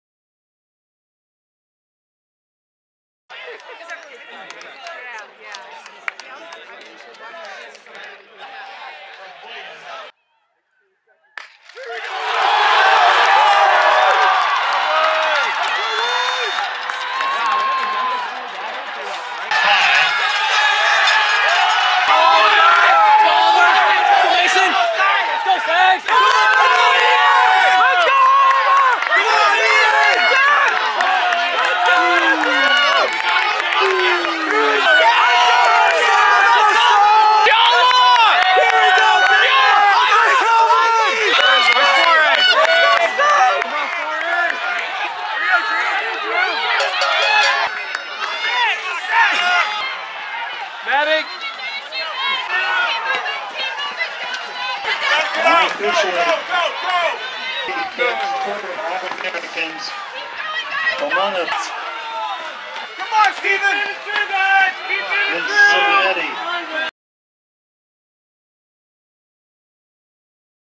CMS Men's Cross Country Highlights
The CMS men's cross country team earned its 15th straight trip to nationals and placed four runners in the top 100 as it earned a 12th place finish on Saturday. Here's a look at some of the action.